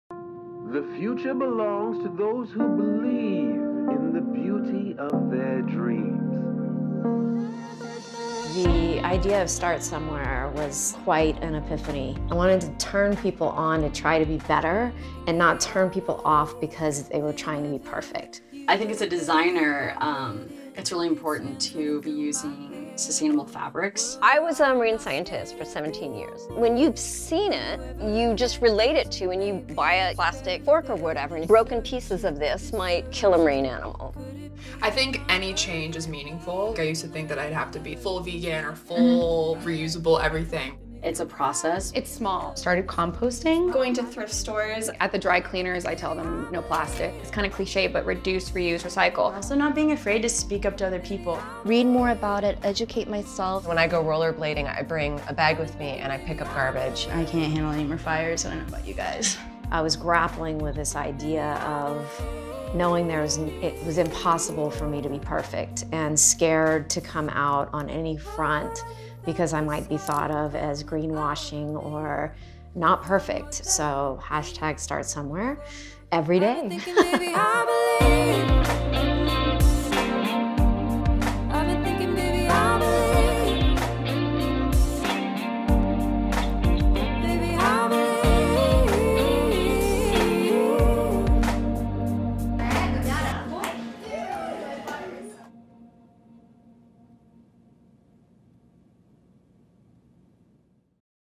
STARTSOMEWHERE By We Are HAH
In Episode 3 of this series by We Are HAH, women discuss how they startsomewhere.

Producers: Roxanna Dunlop & Arden Grier
Director: Arden Grier
Director of Photography: Seannie Bryan
Sound Mixer: Marz Mejia
Production Coordinator: Jeanette Maus
Editor: Arden Grier
Color: Seannie Bryan
Post Sound Mix: Marz Mejia